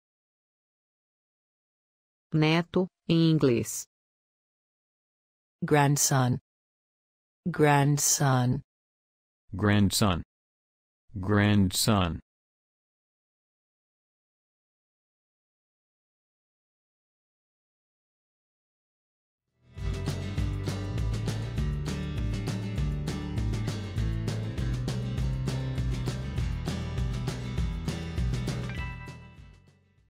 Como dizer neto em inglês
Como se diz corretamente neto em inglês? Ouça conosco.
Qual é a tradução correta da palavra neto do português para o inglês? Fale como os americanos. Além disso, como escrever corretamente a palavra grandson?

Obrigado!